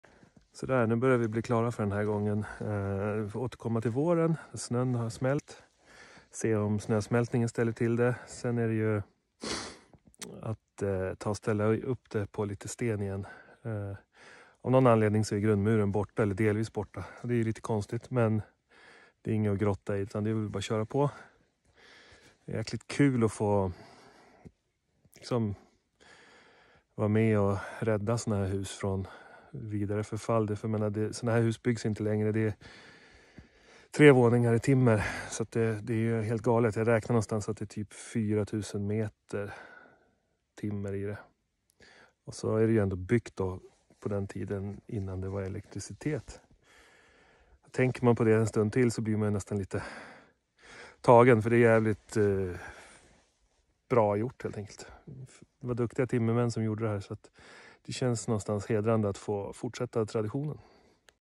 Old log barn restoration. Part 1.
Timrat magasin där grunden fallit bort. Lyft och säkring inför snösmältning. En kort summering av vad vi gjort.